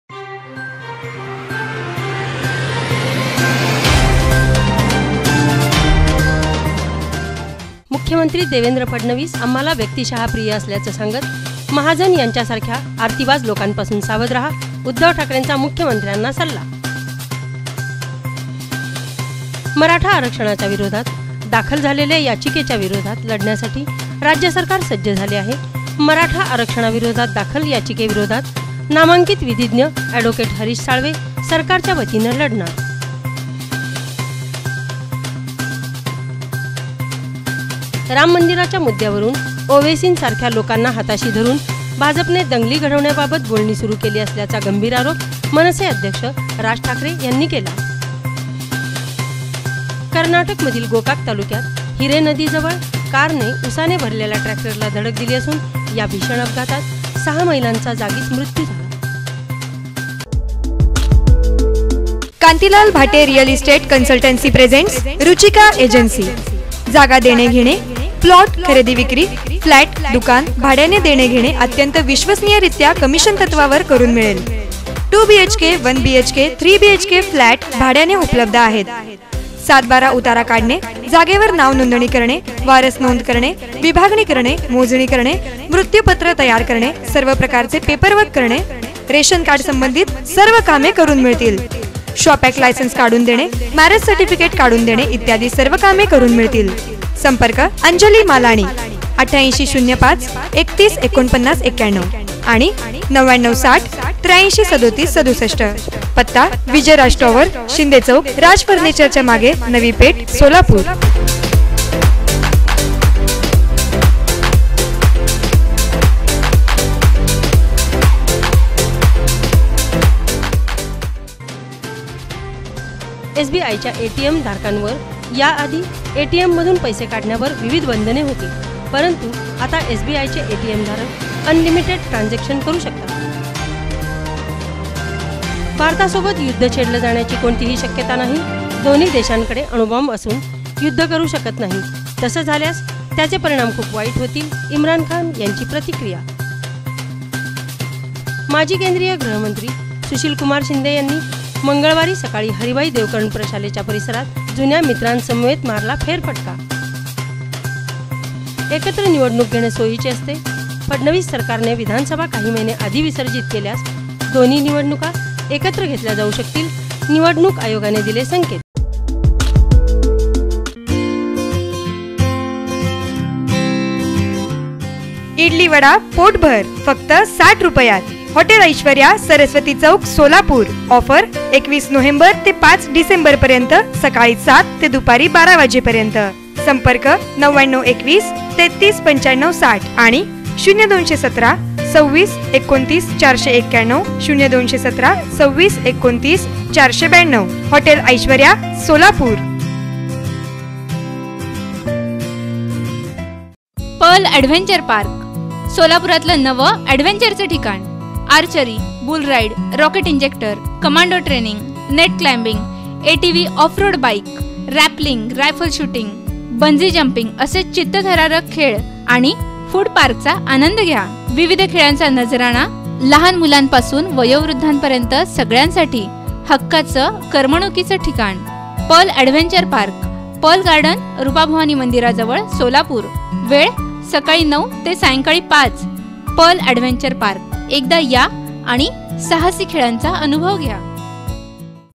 मुख्यमंत्र्यांनी गिरीष महाजनांपासून सावध राहावं : उद्धव ठाकरे
मुख्यमंत्री देवेंद्र फडणवीस आम्हाला व्यक्तिशः प्रिय असल्याचं सांगत, महाजन यांच्यासारख्या 'आरतीबाज' लोकांपासून सावध रहा उद्धव ठाकरेंचा मुख्यमंत्र्यांना सल्ला

मराठा आरक्षणाच्या विरोधात दाखल झालेल्या याचिकेच्या विरोधात लढण्यासाठी राज्य सरकार सज्ज झाले आहे. मराठा अारक्षणाविरोधात दाखल याचिकेविरोधात नामांकित विधिज्ञ अॅड. हरीश साळवे सरकारच्यावतीने लढणार

राम मंदिराच्या मुद्द्यावरुन ओवेसींसारख्या लोकांना हाताशी भाजपने दंगली घडवण्याबाबत बोलणी सुरू केली असल्याचा गंभीर आरोप मनसे अध्यक्ष राज ठाकरे यांनी केला आहे

कर्नाटकमधील गोकाक तालुक्यात हिरे नदीजवळ कारने ऊसाने भरलेल्या ट्रॅक्टरला धडक दिली असून या भीषण अपघातात सहा महिलांचा मृत्यू झाला

एसबीआयच्या एटीएमधारकांवर याआधी एटीएममधून पैसे काढण्यावर विविध बंधने होती. परंतु आता एसबीआयचे एटीएमधारक अनलिमिटेड ट्रान्जॅक्शन करु शकतात. परंतु अनलिमिटेड ट्रान्झॅक्शनसाठी बँकेच्या काही अटींचे पालन करावे लागणार आहे.

भारतासोबत युद्ध छेडलं जाण्याची कोणतीही शक्यता नाही. दोन्ही देशांकडे अणुबॉम्ब असून, युद्ध करु शकत नाही. तसं झाल्यास त्याचे परिणाम खूप वाईट होतील इम्रान खान यांची प्रतिक्रिया

माजी केंद्रीय गृहमंत्री सुशीलकुमार शिंदे यांनी मंगळवारी सकाळी हरिभाई देवकरण प्रशालेच्या परिसरात जुन्या मित्रांसमवेत फेरफटका मारला

एकत्र निवडणूक घेणे सोयीचे असते. फडणवीस सरकारने विधानसभा काही महिने आधी विसर्जित केल्यास दोन्ही निवडणुका एकत्र घेतल्या जाऊ शकतील, निवडणूक आयोगाने दिले संकेत